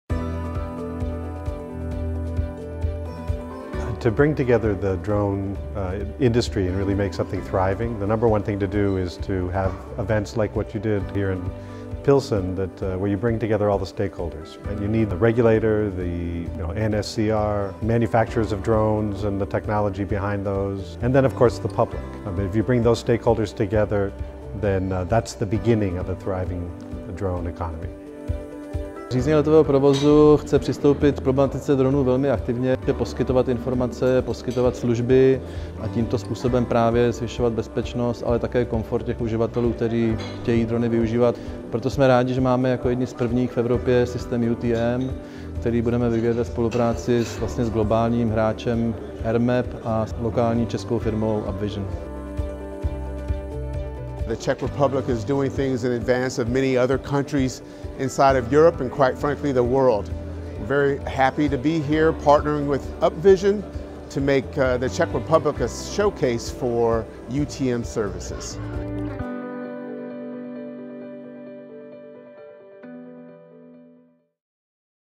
U-Space demonstration in the Czech Republic (EN version)
U-Space demonstration in the Czech Republic during the 4th conference of the Czech Unmanned Aerial Alliance (UAVA) in Pilsen city.